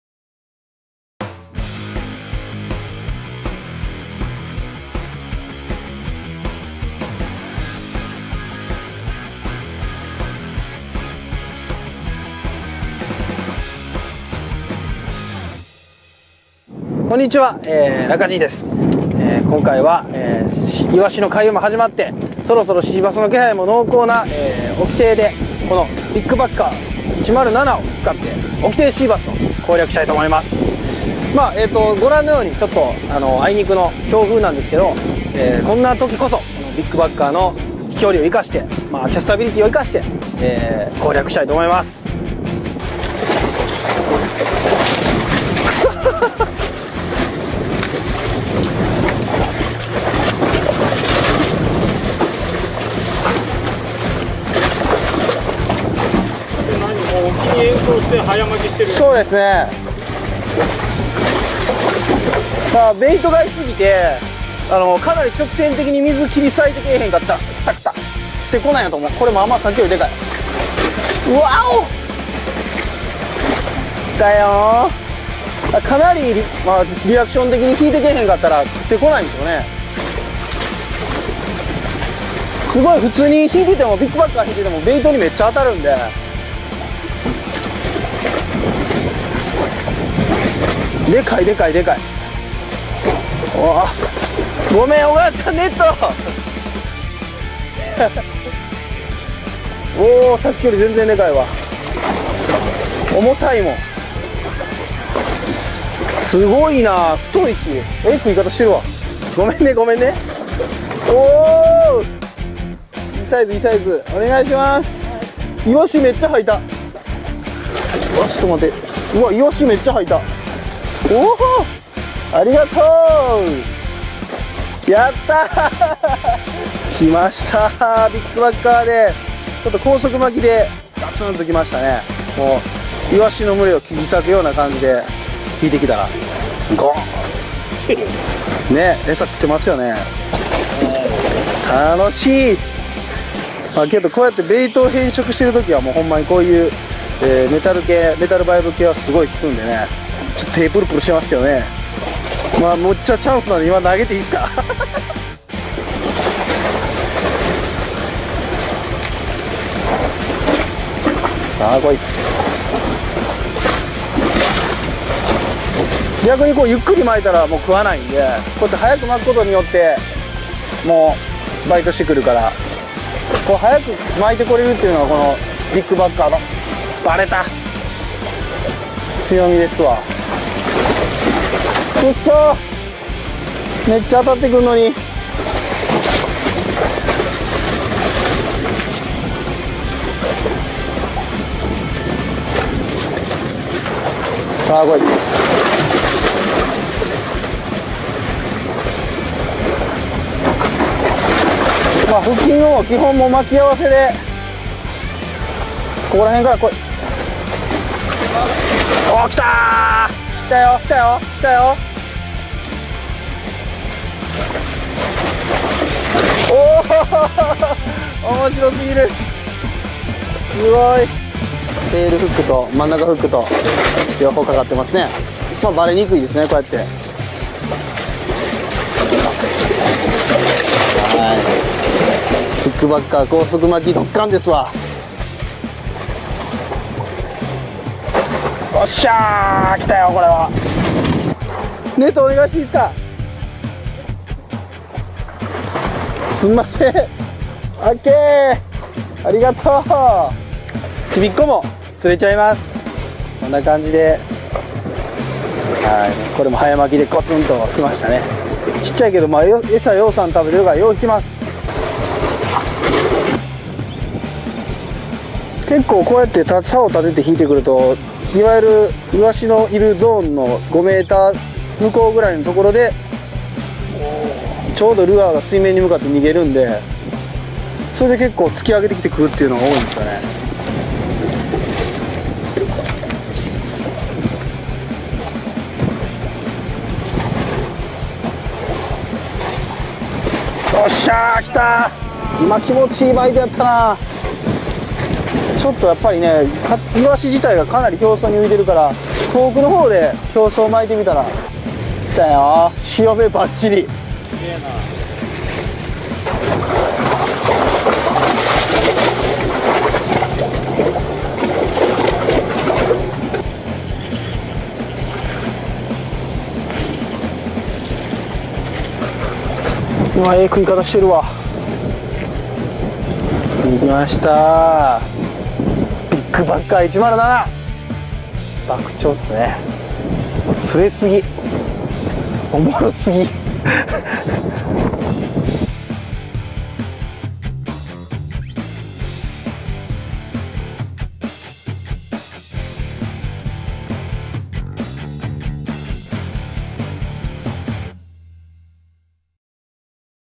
2012.07.12 ビッグバッカー、いざ！
新作シーバス用メタルバイブ「ビッグバッカー」の威力をナカジーが見せます！